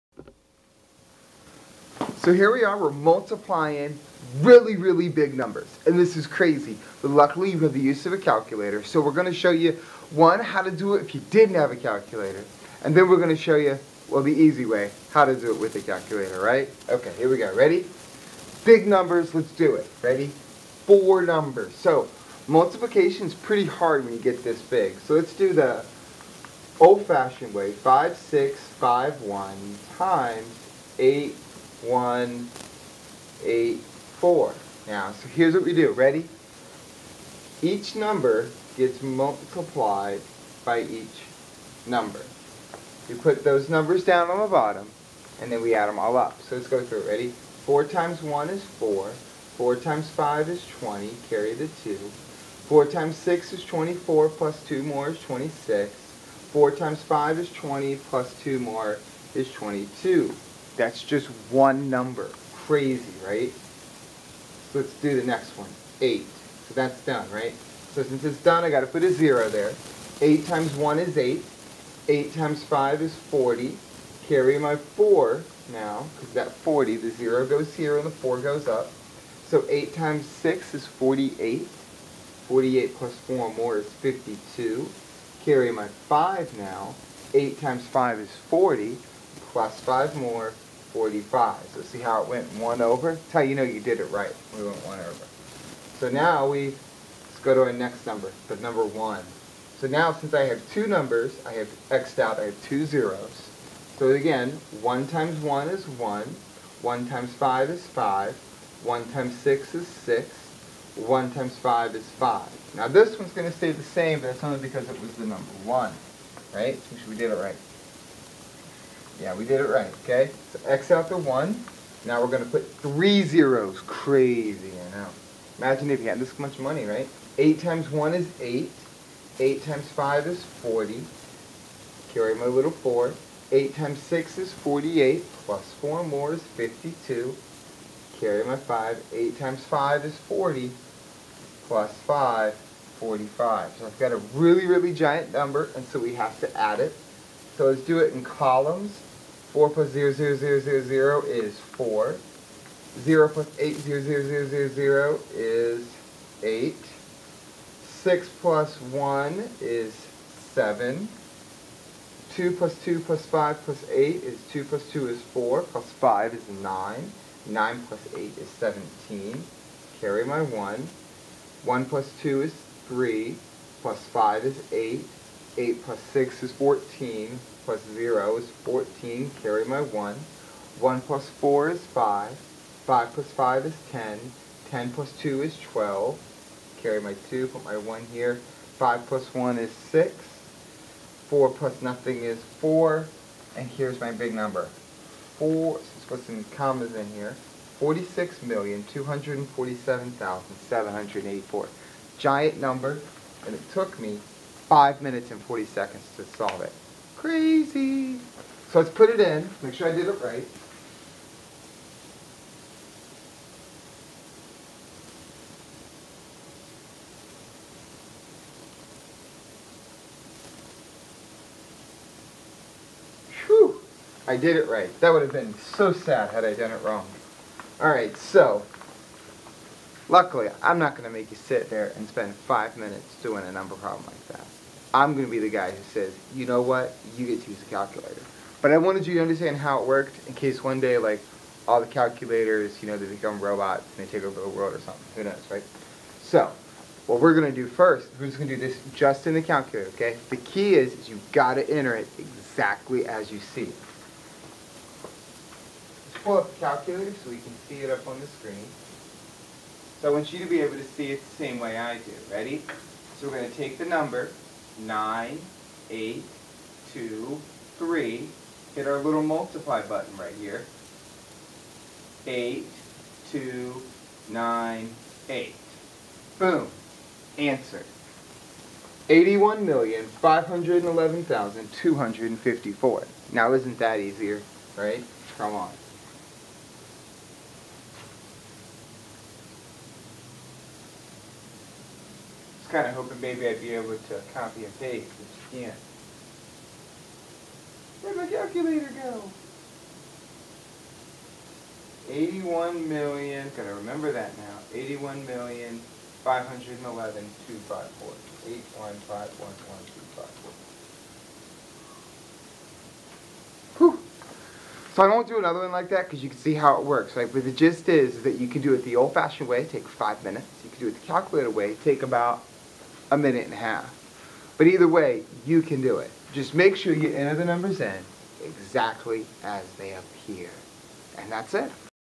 Multiplying whole numbers with 4 or more digits.
Watch a video review of how to multiply 4 digit multiplication with and without the use of calculator.